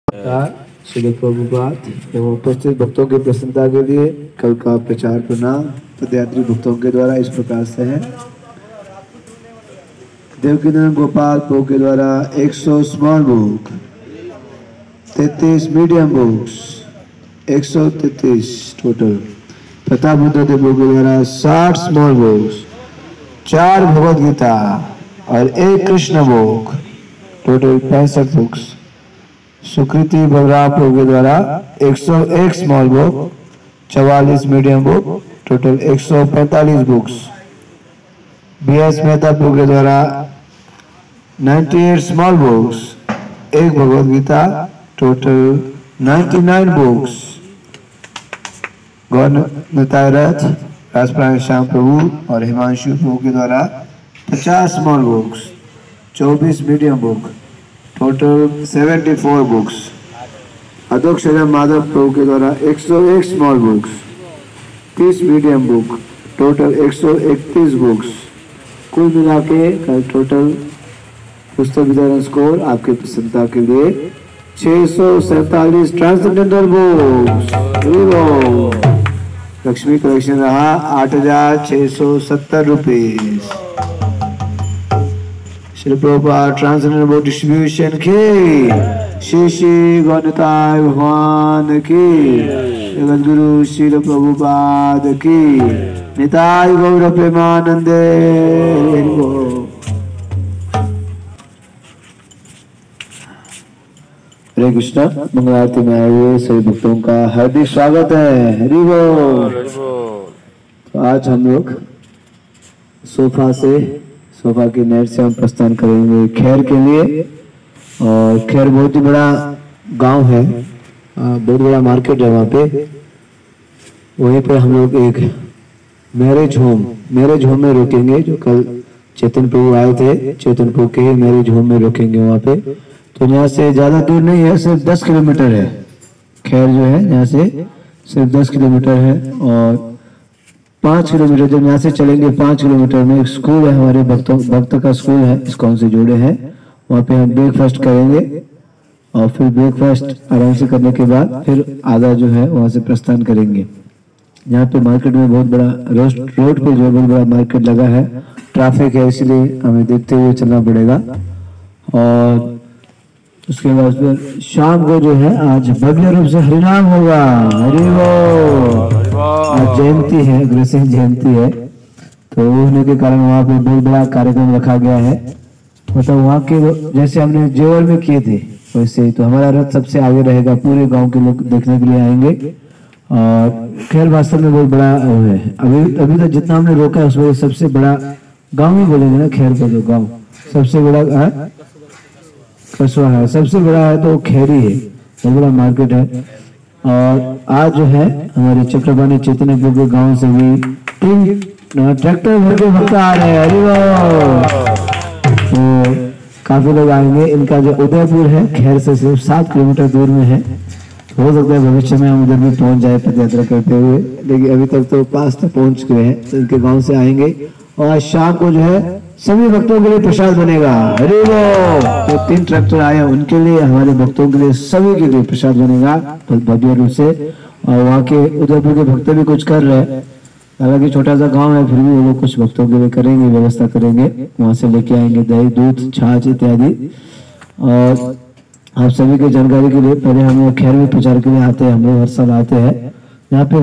PadaYatra 10th day's Mangal Arati's announcement of Book's distribution. 12 October 2021.
Iskcon Padaytra
भगवान की दिव्य नाम का प्रचार-प्रसार
हरि हरि वाले, नाचिए गाइए

Padaytra Noida to Vrindavan 2021

Channels owner
Chaitanya Kinkar Das